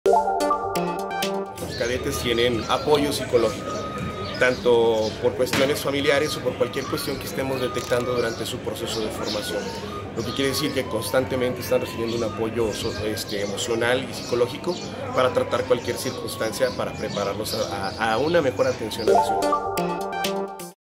Tras ataques en contra de policías, cadetes siguen en formación
ACADEMIA CADETES INS